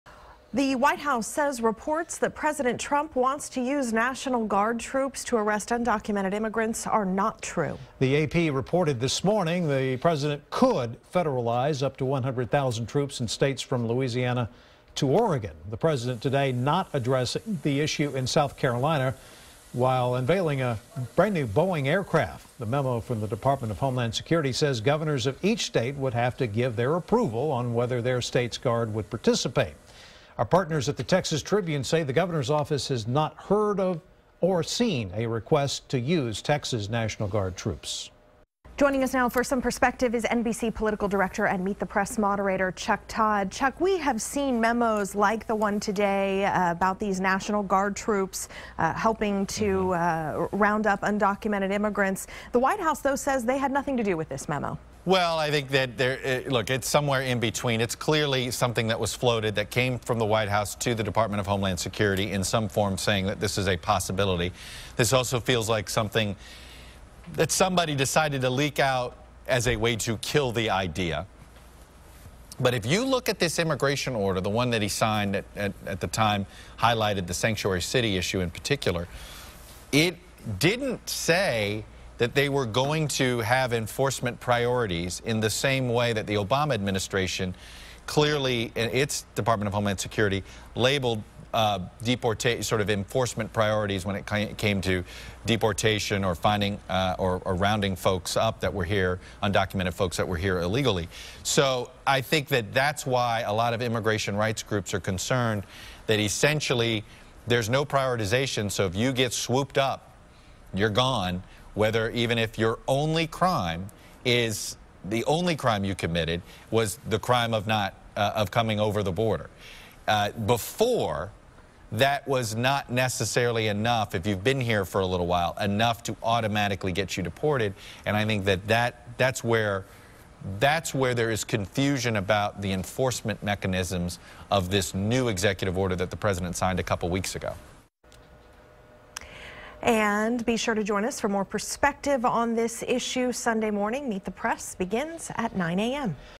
Trump administration says National Guard plan not seriously considered
The White House says there is no effort to use the National Guard to round up “illegal immigrants.”